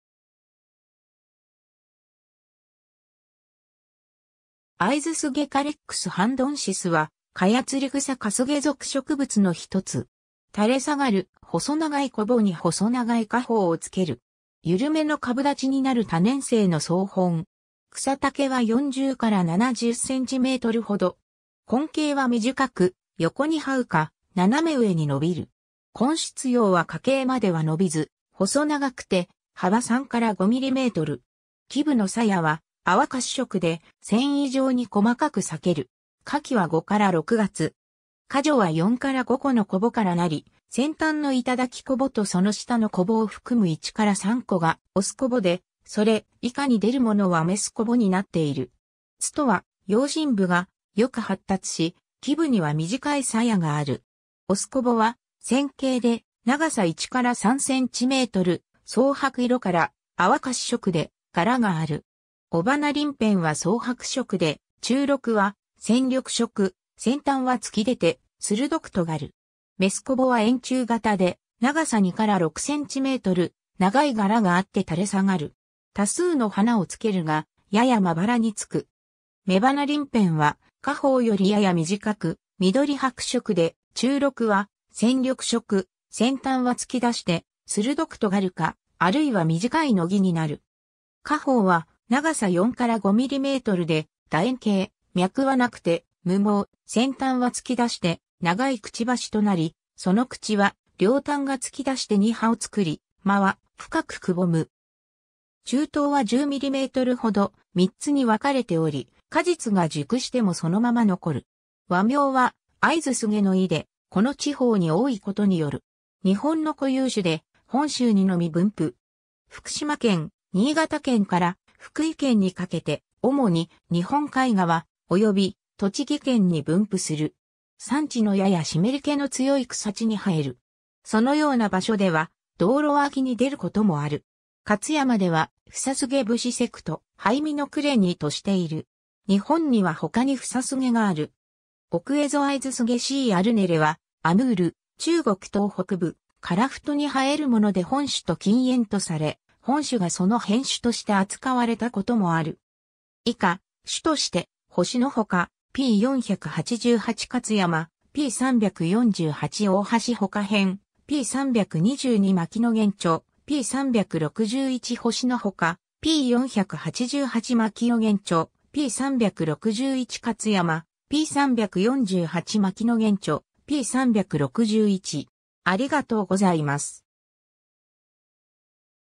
アイズスゲ
アイズスゲ Carex hondoensis はカヤツリグサ科スゲ属植物の1つ。
垂れ下がる細長い小穂に細長い果胞をつける。
 緩めの株立ちになる多年生の草本。
草丈は40-70cmほど。
根茎は短く横に這うか、斜め上に伸びる。
根出葉は花茎までは伸びず、細長くて幅3-5mm。
基部の鞘は淡褐色で、繊維状に細かく裂ける。
花序は4-5個の小穂からなり、先端の頂小穂とその下の小穂を含む1-3個が雄小穂で、それ以下に出るものは雌小穂になっている。
苞は葉身部がよく発達し、基部には短い鞘がある。
雄小穂は線形で長さ1-3cm、蒼白色から淡褐色で柄がある。
雄花鱗片は蒼白色で中肋は鮮緑色、先端は突き出て鋭く尖る。
雌小穂は円柱形で長さ2-6cm、長い柄があって垂れ下がる。
多数の花を着けるが、ややまばらに着く。
雌花鱗片は果胞よりやや短く、緑白色で中肋は鮮緑色、先端は突き出して鋭く尖るか、あるいは短い芒になる。
果胞は長さ4-5mmで楕円形、脈はなくて無毛、先端は突き出して長い嘴となり、その口は両端が突き出して2歯を作り、間は深くくぼむ。
柱頭は10mmほど、3つに分かれており、果実が熟してもそのまま残る。
 和名は会津スゲの意で、この地方に多いことによる。
 日本の固有種で本州にのみ分布。
福島県、新潟県から福井県にかけて、主に日本海側、および栃木県に分布する。
 山地のやや湿り気の強い草地に生える。
そのような場所では道路脇に出ることもある。
 勝山(2015)ではフサスゲ節 Sect. Hymenochlaenae としている。
日本には他にフサスゲがある。
 オクエゾアイズスゲ C. arnellii はアムール、中国東北部、樺太に生えるもので本種と近縁とされ、本種がその変種として扱われたこともある。